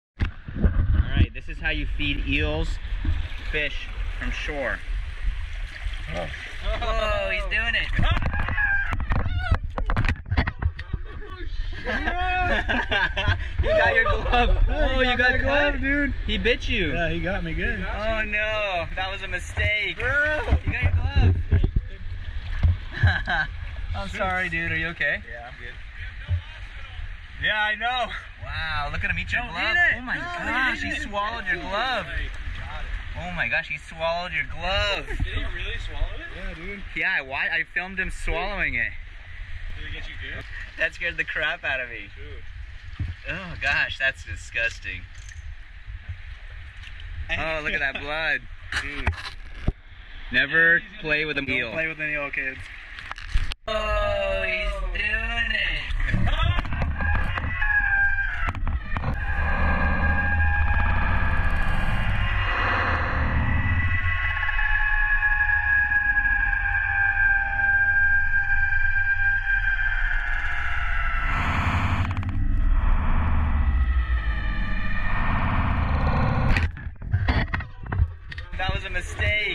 EEL BITES HAND ON LAND!!
Other titles that were considered:

How NOT to Feed an Eel
Eel Bites the Hand that Feeds It
Man Vs. Eel, Gloves Off
Man Screams at Flying Eel
Failed Fishing Finds Finger Fanged
A Mistake Was Made

Which title do you like?